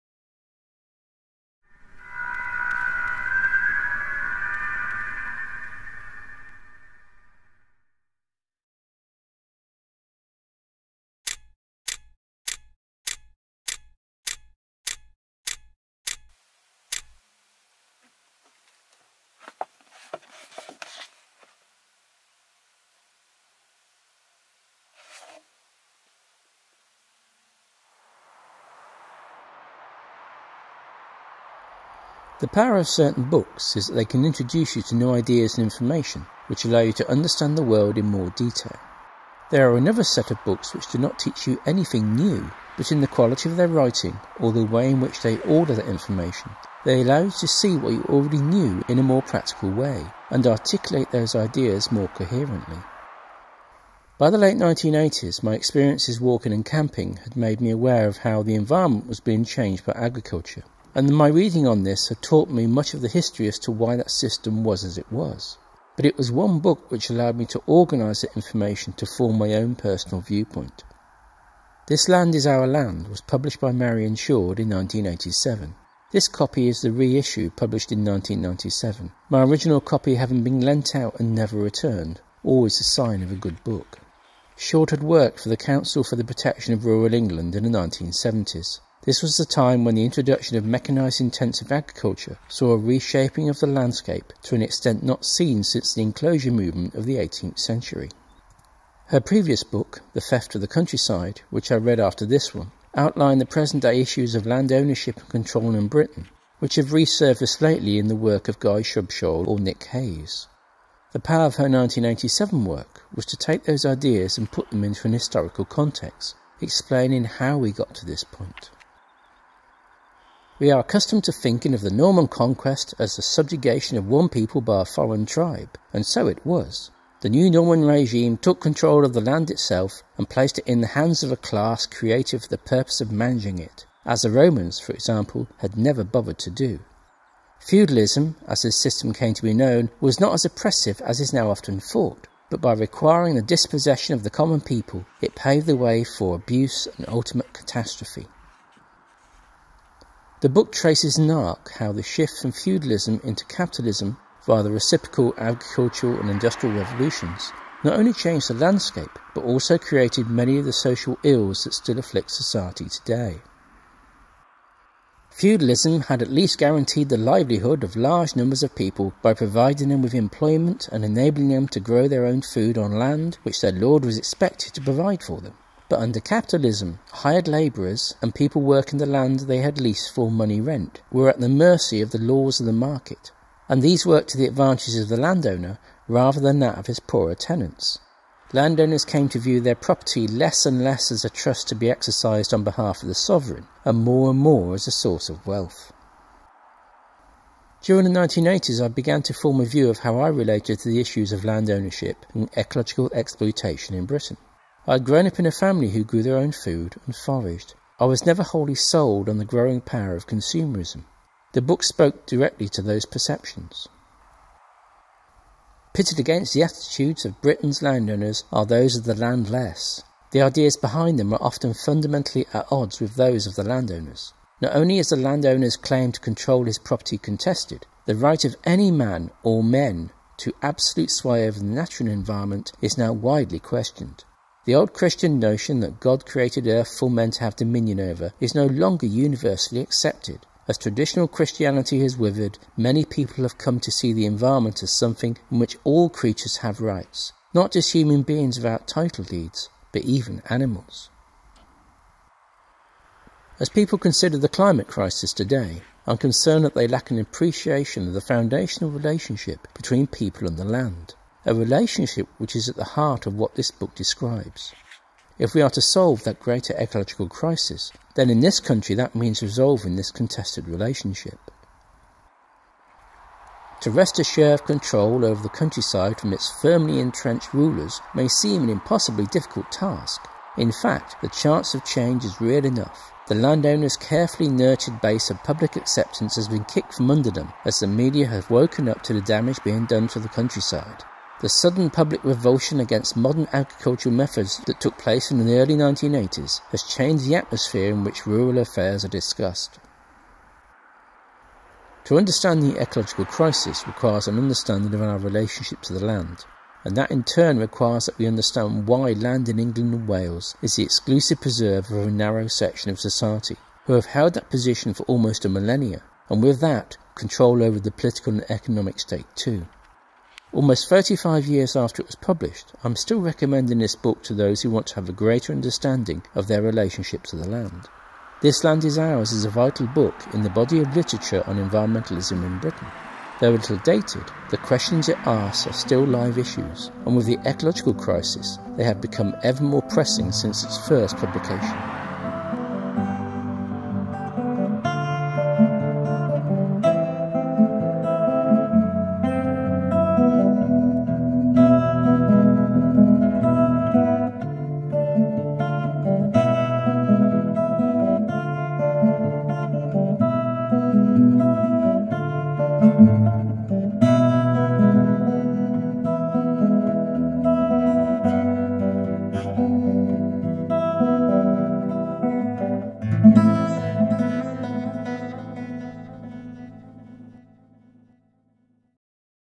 ‘This Land is Our Land’, Marion Shoard (1987) – A Book in Five Minutes, No.6
Ramblinactivist 2021/24, 7th November 2021

“To wrest a share of control over the countryside from its firmly entrenched rulers may seem an almost impossibly difficult task... In fact, the chance of change is real enough.”

Thirty-five years ago Marion Shoard published a book which still stands as one of the landmark books documentaing the history of English land rights -- and the urgency for a change in the present patterns of land ownership and exploitation in Britain which have endured since the ‘dispossession’ of the land almost 1,000 years ago.

In today’s digital analytics popularity contest, all that button-pressing means something in this messed-up world.